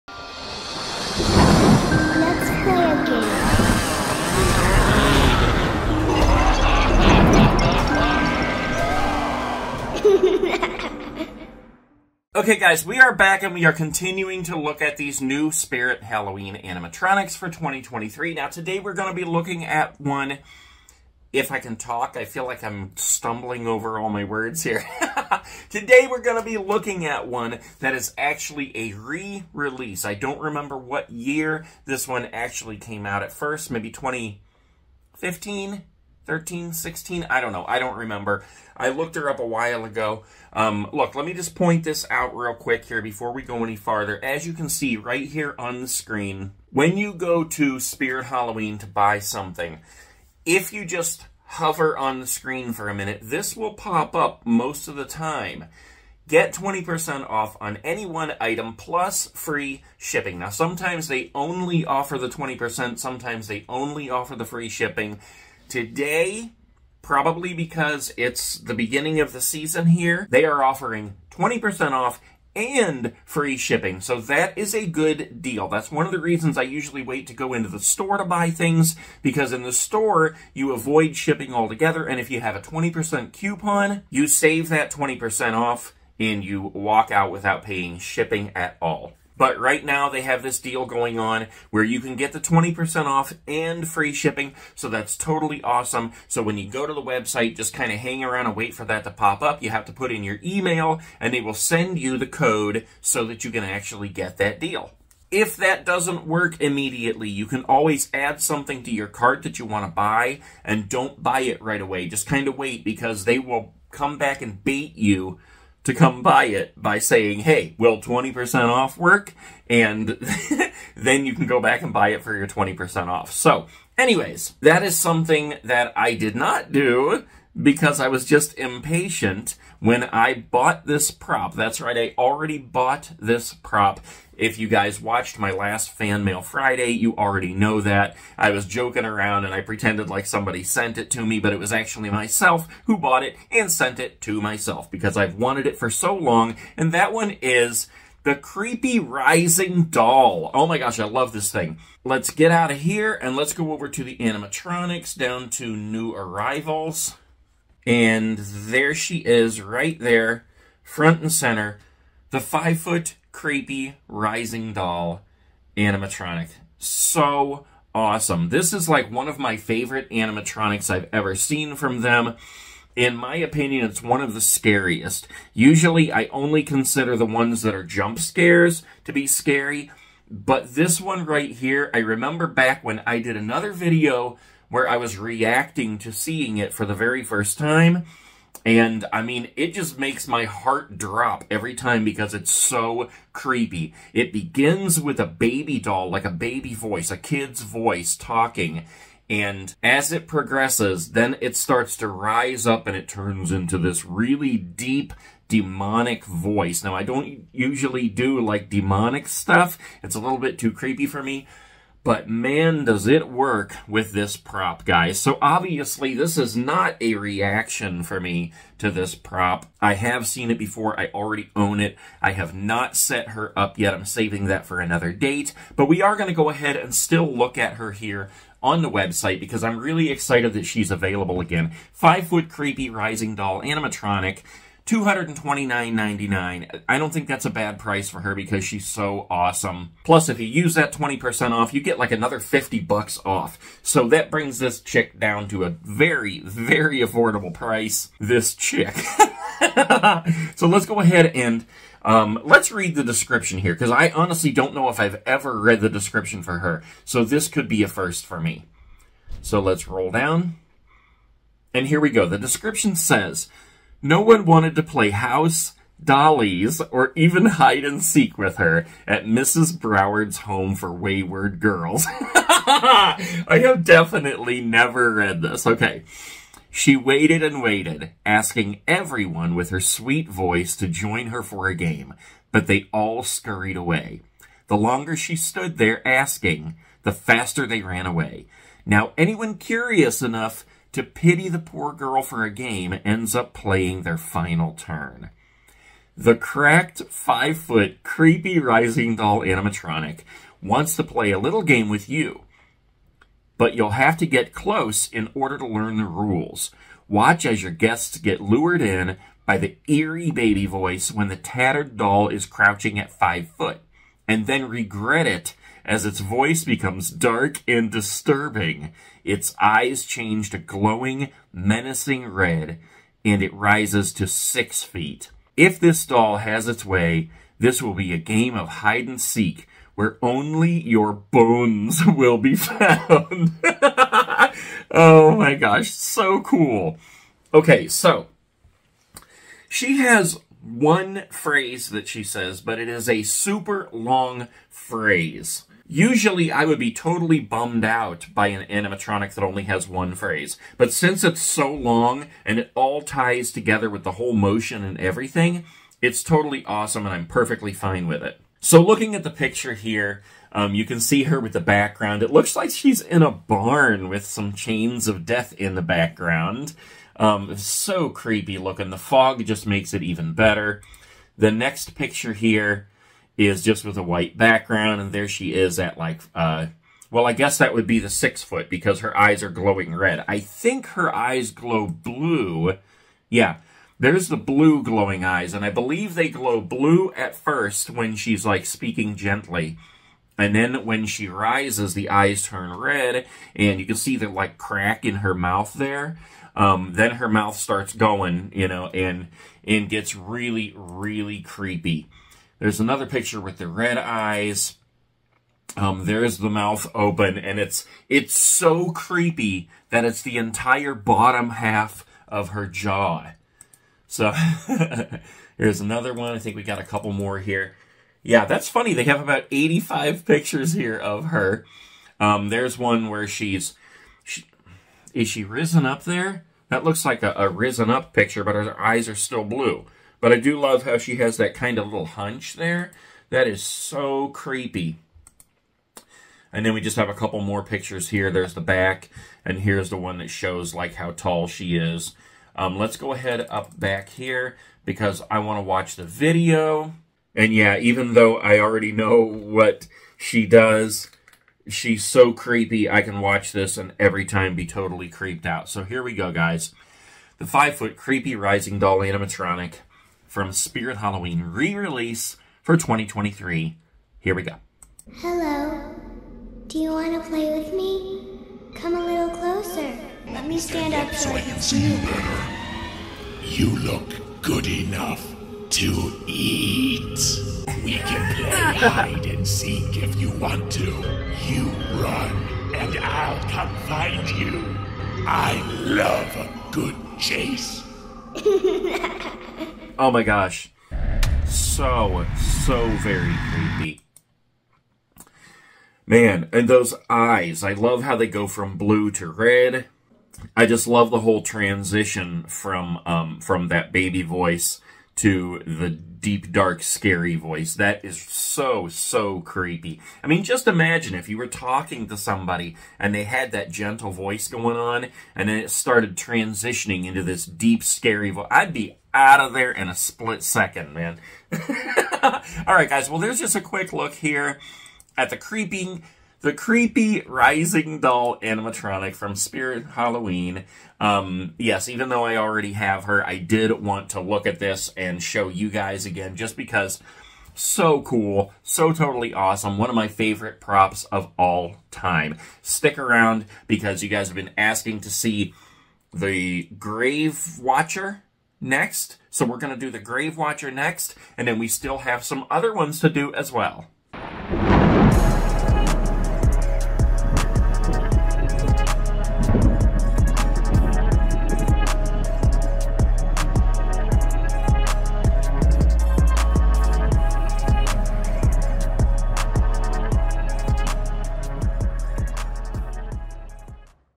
SPIRIT HALLOWEEN 2023 CREEPY RISING DOLL ANIMATRONIC REACTION REVEAL SNEAK PEEK SPIRIT HALLOWS
THE CREEPY RISING DOLL !! Today we are taking a look at the re-release of the Creepy Rising Doll animatronic from Spirit Halloween for the 2023 season. She is SCARY SPOOKY and CREEPY all in one!!

SEND FAN MAIL TO NATE!
        That Nate Guy On YouTube